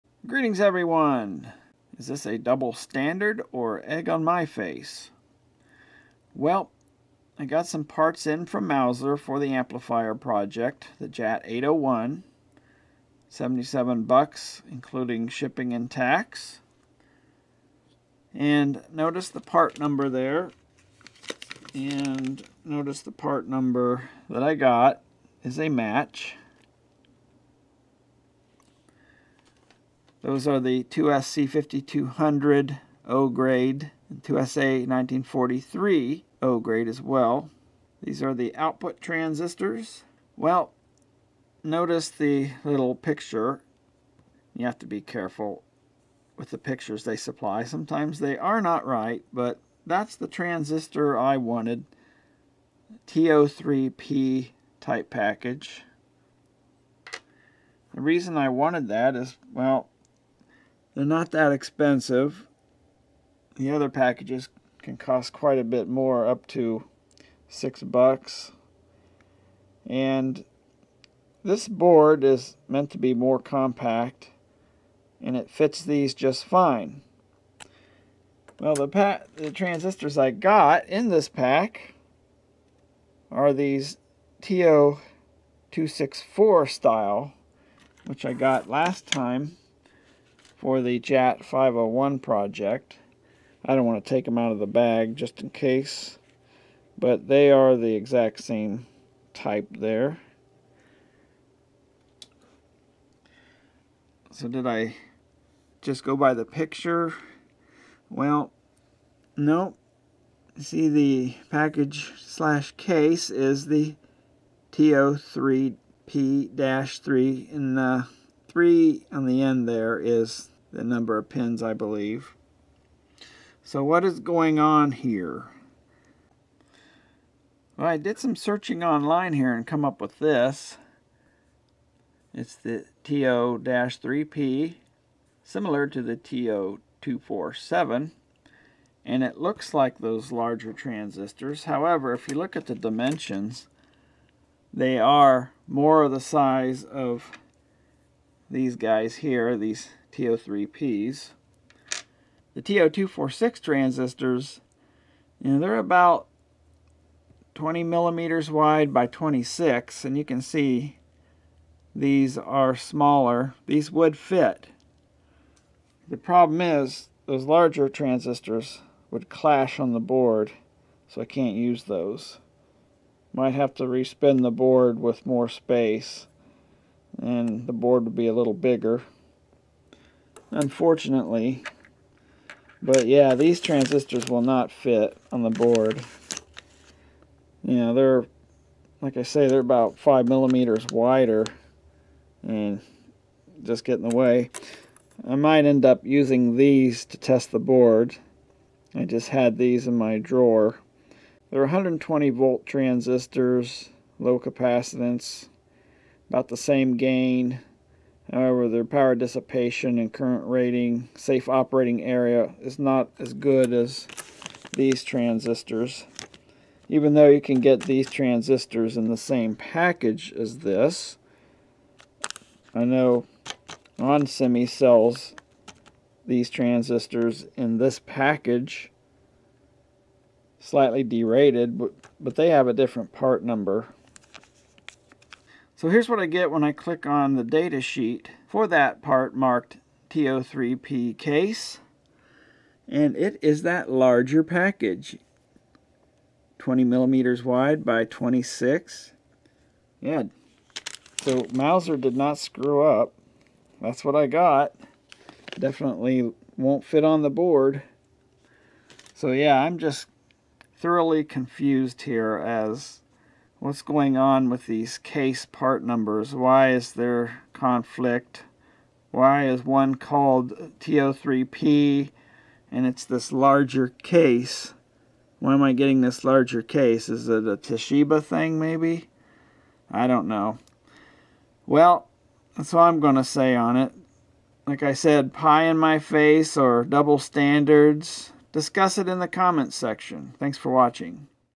Transistor case style confusion
Planning to assemble the JAT801 amplifier board but I got transistors that won't fit!